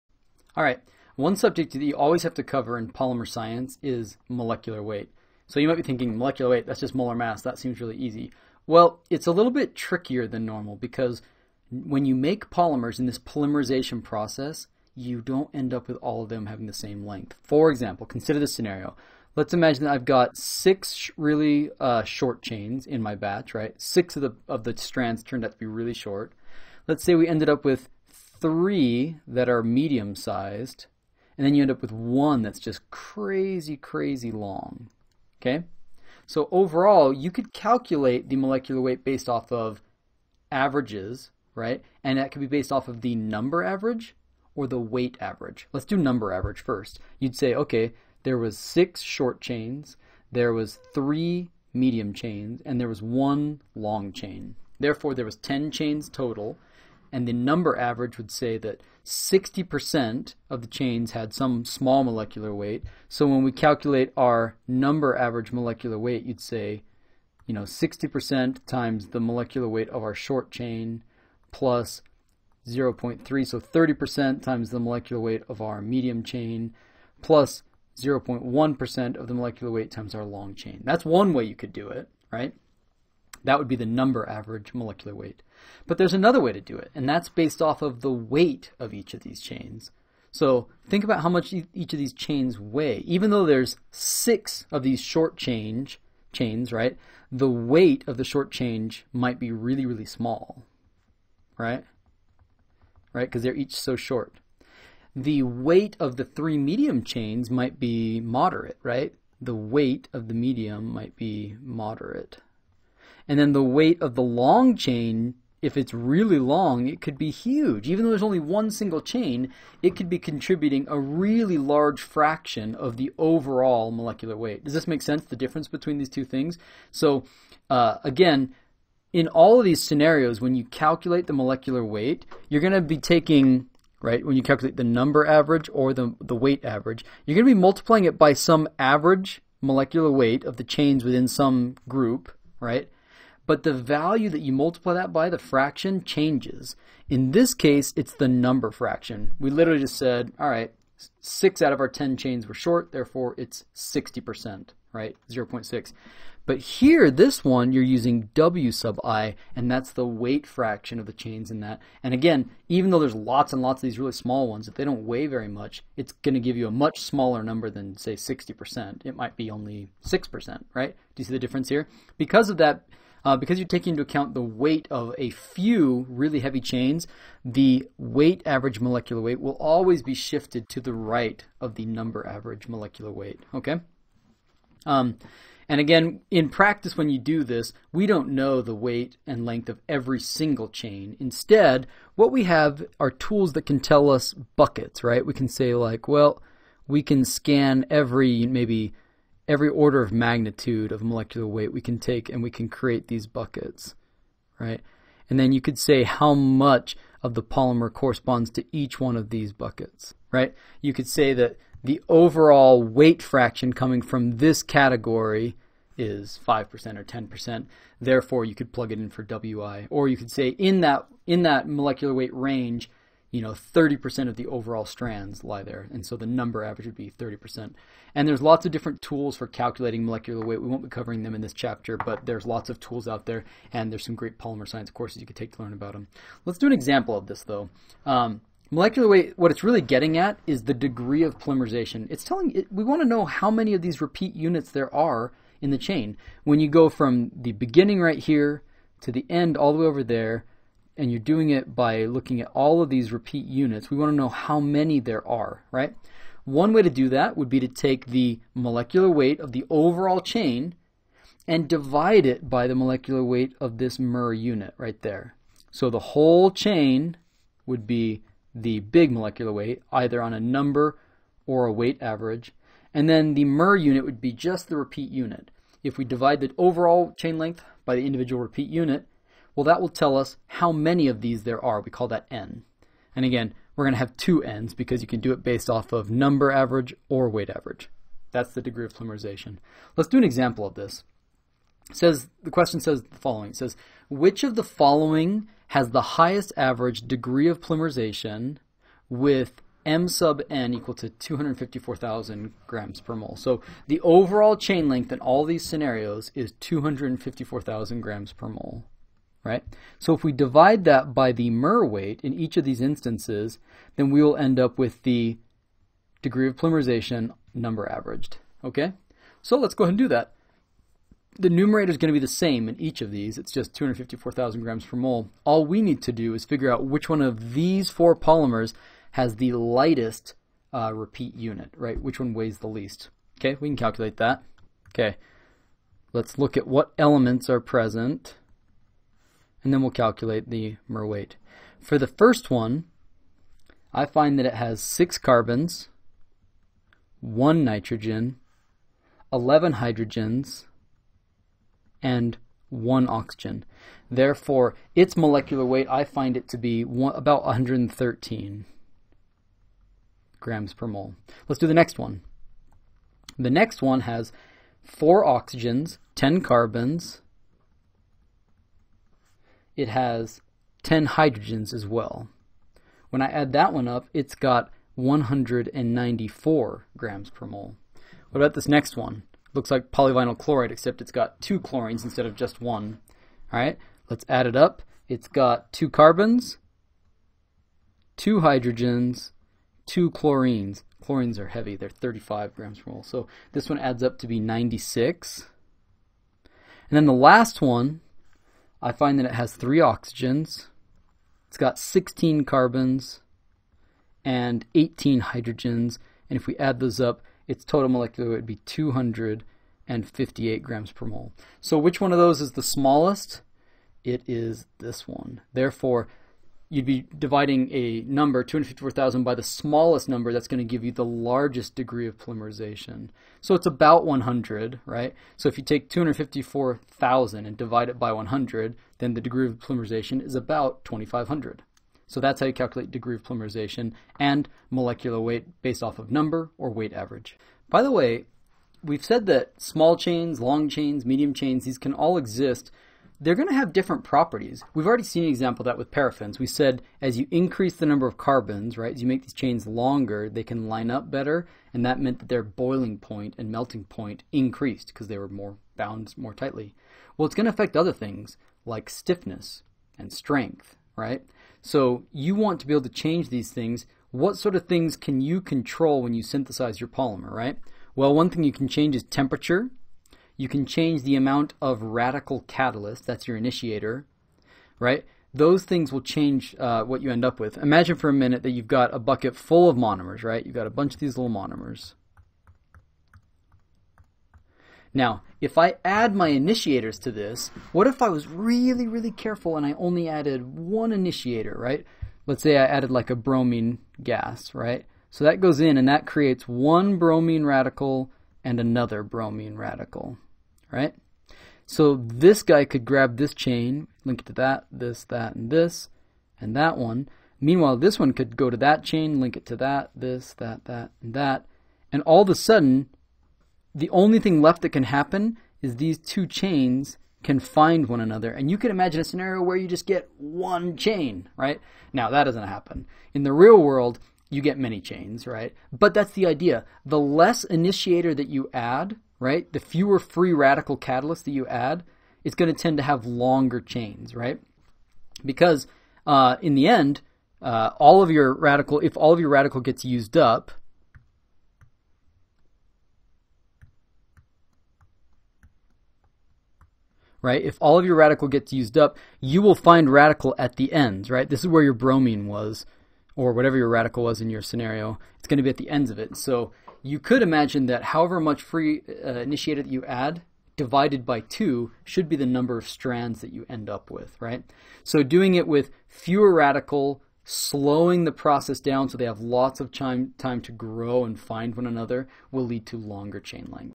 Calculating Molecular Weight (number and weight average) for polymers
Molecular weight is an important for polymers. Since polymer processing typically produces a distribution of different chain lengths, both short and long, we need a tool that calculates the average molecular weight. We can do this based on number average or weight average. Degree of polymerization can then be calculated to determine the number of mer units along the polymer chain based on average molecular weight (number or weight averaged).